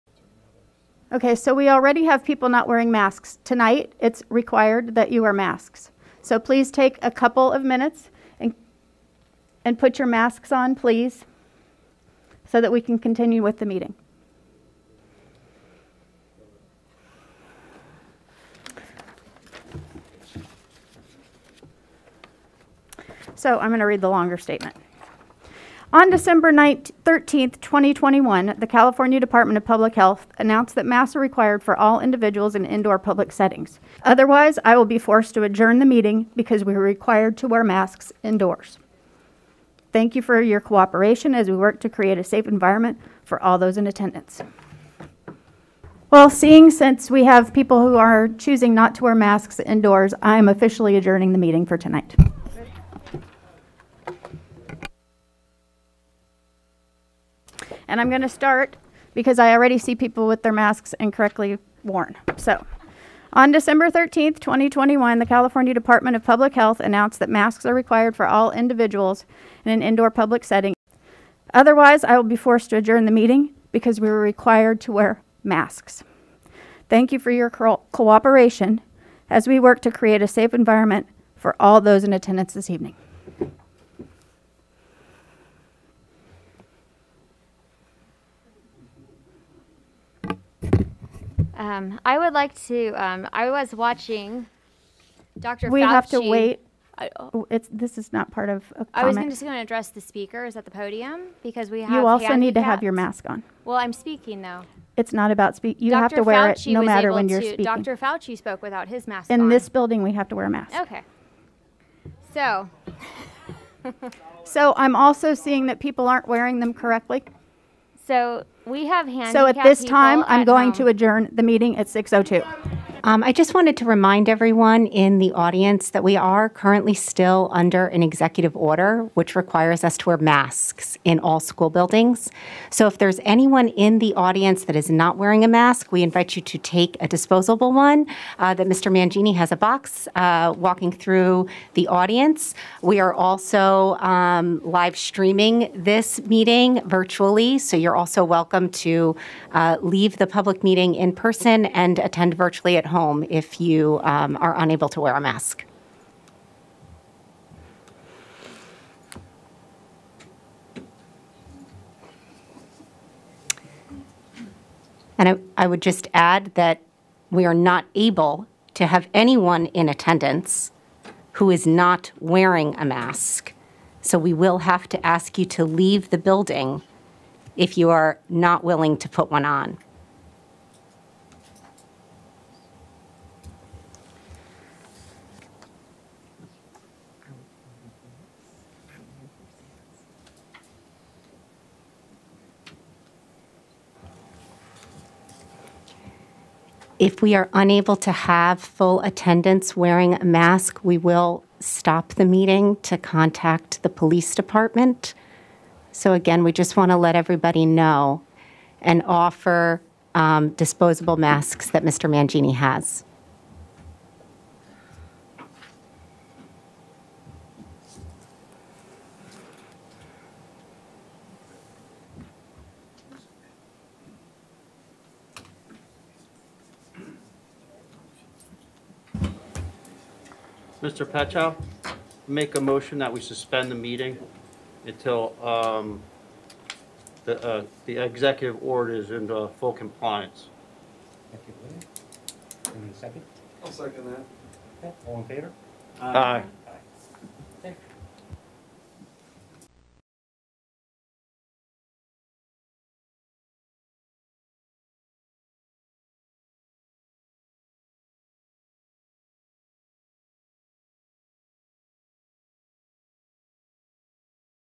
School Board Cancels Meeting Due To Masks Two Weeks In A Row
The Placentia-Yorba Linda Unified School District in California can't seem to get a school board meeting going when people won't mask to the president's satisfaction.